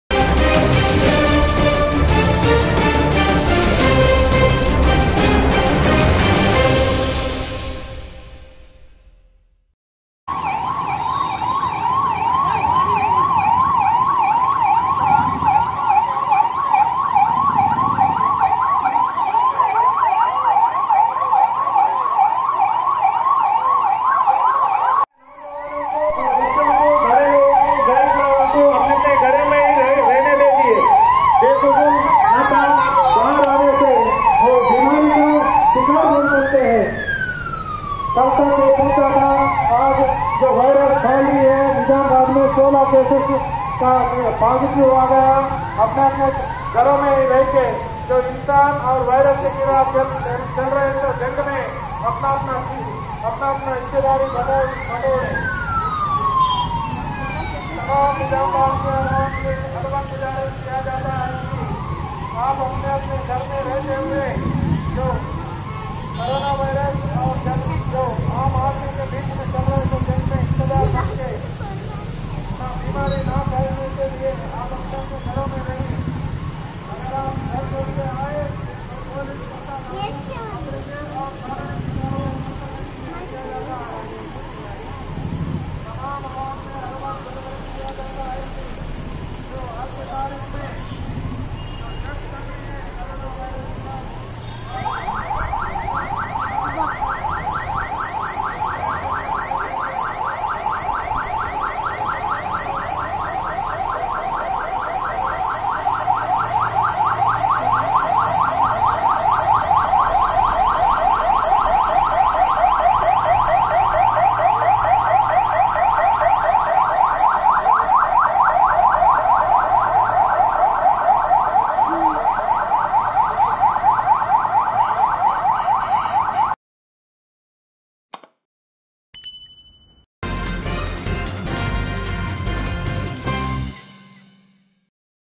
NIZAMBAD ME POLICE KI BADE PAYMANE PAR PATROLLING
NIZAMBAD ME POLICE KI BADE PAYMANE PAR PATROLLING | AWAAM SE GHAROU ME REHNE BAHAR NA NIKALNE KI APPEAL.